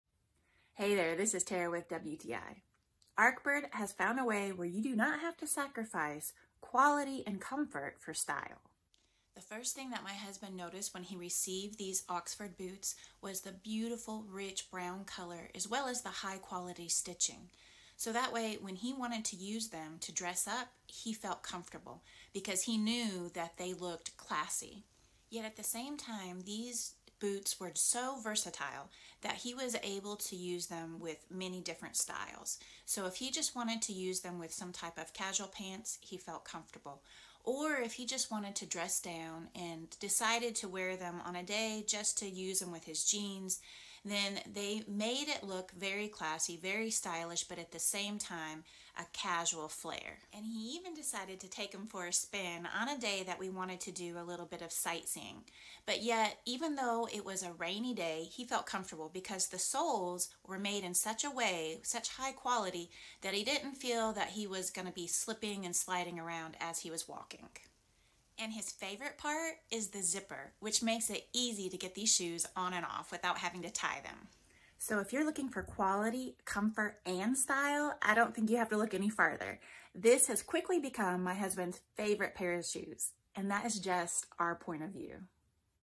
Arkbird Men's Oxford Dress Boots | Our Point Of View
Arkbird Men's Oxford Dress Boots

(Commissionable Links)

Sleek and Stylish Arkbird lace-up oxford dress boots with side zip is the perfect way to add some fashion style to your every day look.
Premium full grain leather upper , soft and comfortable, matching with breathable cushioned leather insole keep your feet dry and odor-free through the day.
The Arkbird leather boots aren't just about the fit. We make sure they look good down to the smallest details.
Construction Details : Durable rubber sole with non-skid tread for extra grip and traction on all surface.